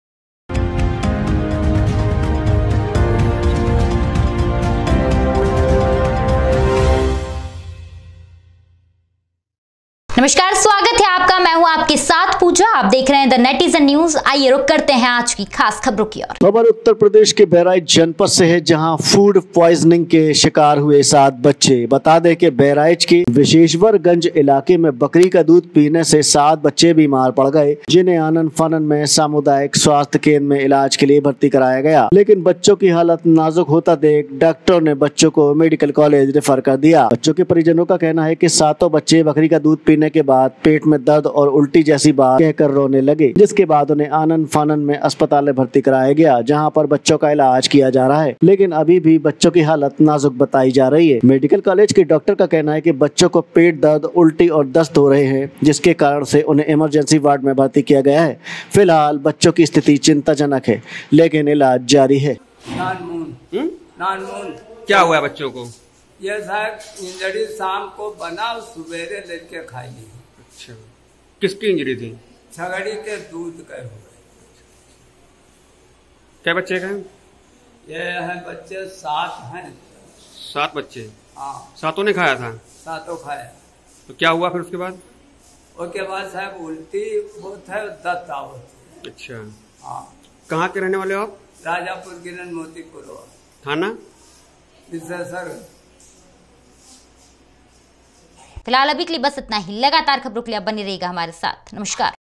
बहराइच : 7 बच्चे हुए फूड प्वाइजनिंग के शिकार
देख डॉक्टरों ने बच्चों को मेडिकल कॉलेज रेफर कर दिया है. बच्चों के परिजनों का कहना है कि सातों बच्चे बकरी का दूध पीने के बाद पेट में दर्द और उल्टी जैसी बात कह कर रोने लगे जिसके बाद उन्हें आनन-फानन में अस्पताल में भर्ती करवाया गया